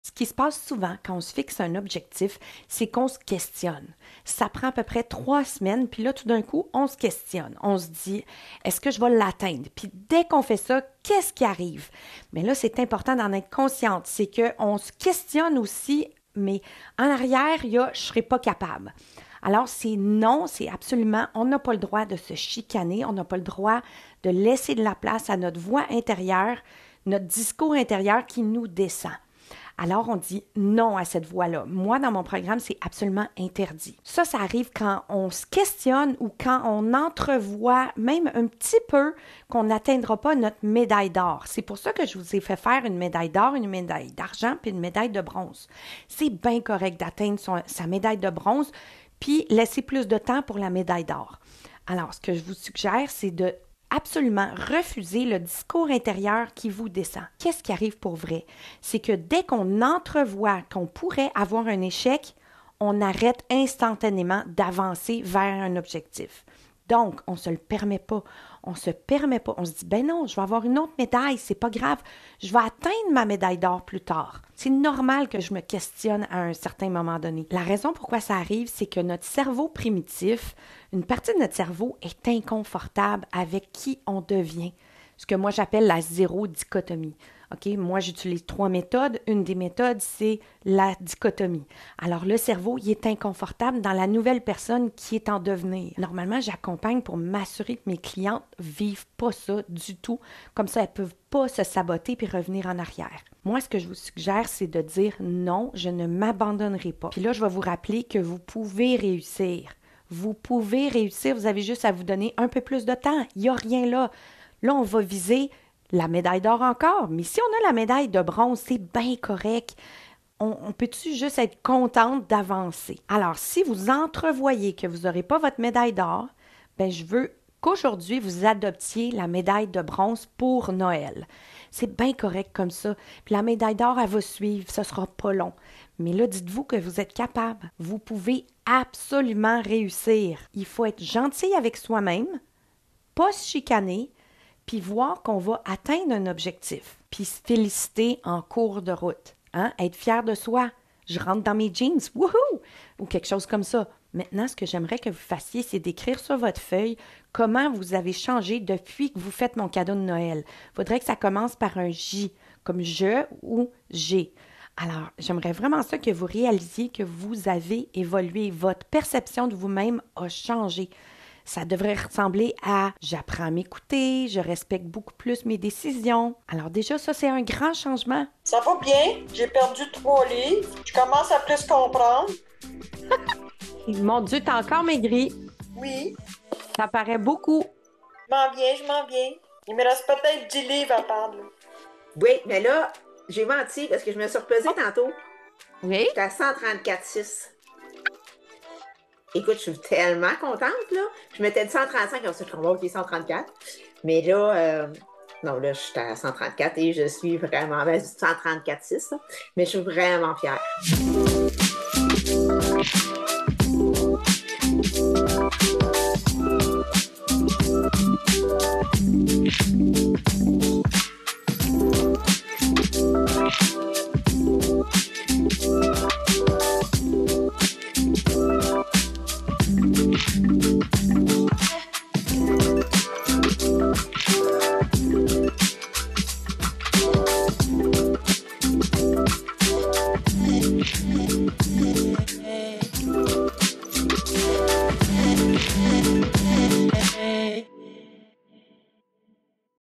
La vraie solution pour perdre du poids
La vraie solution pour perdre du poids est Pour de bon, une méthode facile et durable. Comment maigrir facilement et durablement? POURDEBON fonctionne sans sport. Je suis coach en perte de poids et mon cadeau des fêtes: perdez avec moi Sophie votre coach.  Je ne ferai pas de déconditionnement ici car vous avez besoin d'encadrement pour ça mais vous perdrez quand même du poids d'ici Noël juste à faire le travail que je vous propose :) dans cette série